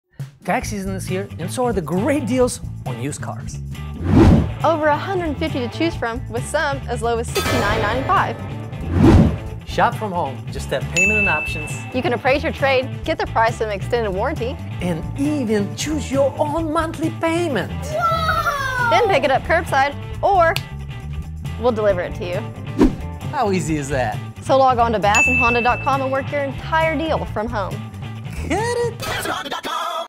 Great Deals at Battison Honda OKC
Tax season is here and so are the great deals on used cars!
Browse our inventory of over 150 pre-owned vehicles right from your couch. You can even work the entire deal without stepping a foot into the dealership.

Battison Honda
8700 Northwest Expressway
OKC, OK 73162